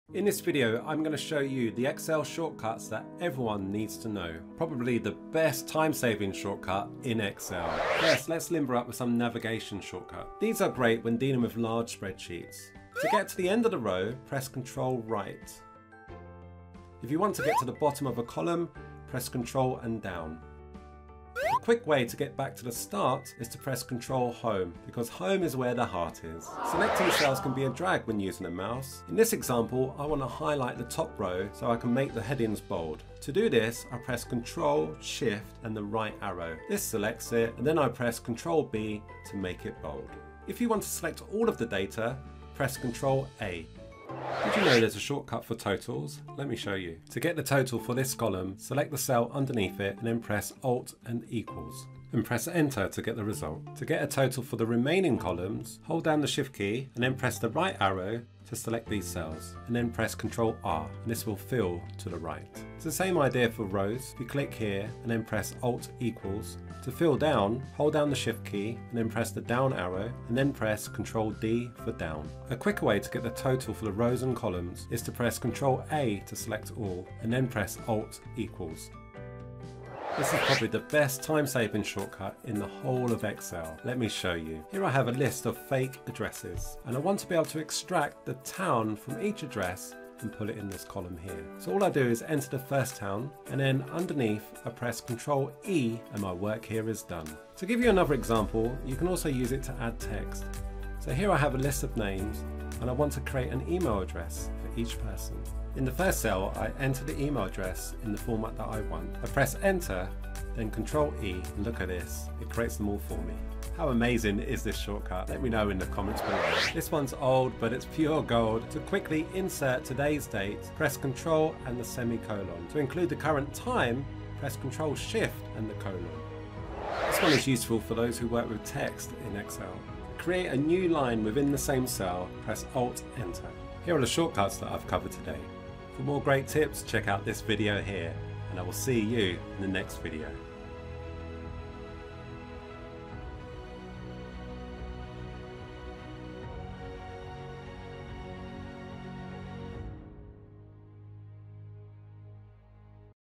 Excel Keyboard Shortcuts EVERYONE should know
In this video I will show you the Excel Keyboard Shortcuts EVERYONE needs to know. These are the shortcuts you will need on a daily basis. Did you know there is a keyboard shortcut to sum columns and rows? I will also show you the key navigation and selection shortcuts in Excel. There are shortcuts for everything in Microsoft Excel. Knowing the key ones can help you work more efficiently. They can also help you to reduce repetitive strain injuries as you're not continuously relying on the mouse. Learn how to insert the current date and time. This video also covers probably the best time saving shortcut in the whole of Excel. How many of these did you know already?

Time Stamps
0:00 Excel Keyboard Shortcuts EVERYONE should know
0:08 Excel Navigation Shortcuts
0:26 Excel Selection Shortcuts
0:44 Excel Autosum Shortcut
1:20 Excel Flash Fill Shortcut
1:57 Excel Insert Current Date and Time Shortcut
2:07 Excel Insert New Line within a Cell